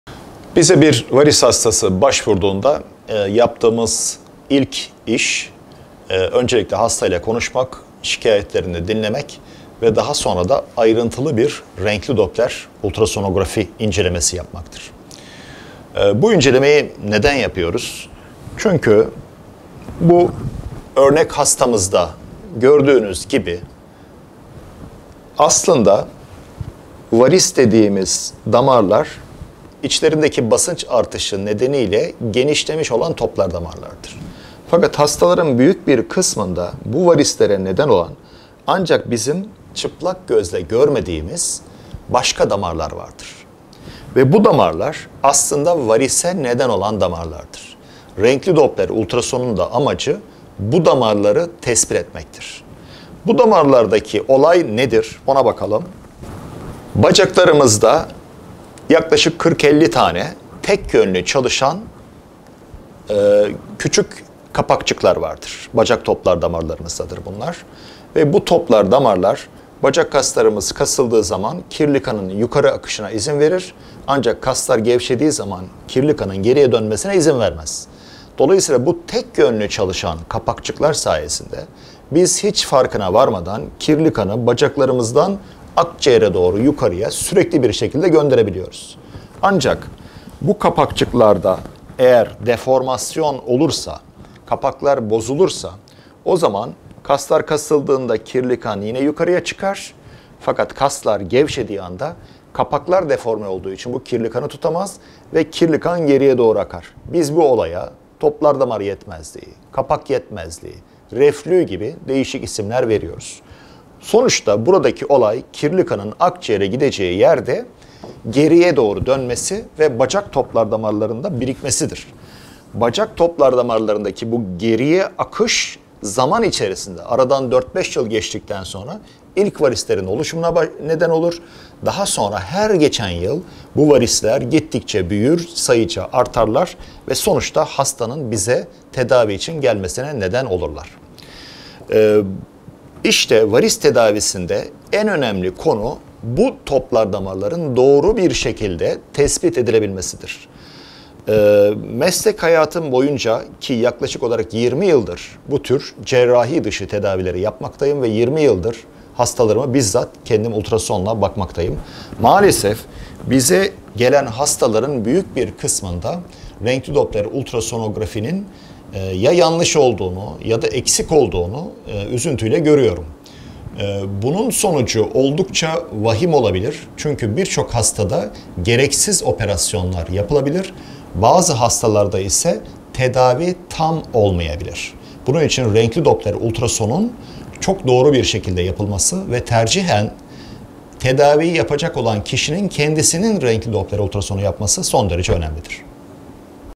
DOPPLER ULTRASON VARİSTE NEDEN ÖNEMLİ?
Varis hastalarında renkli Doppler ultrason incelemesi genellikle yanlış ya da eksik yapılabilmektedir. Bu durum yapılacak tedaviyi de yanlış yönlendirebilir ve hastanın tedavi edilememesine ya da yanlış ya da eksik tedavi görmesine yol açabilir.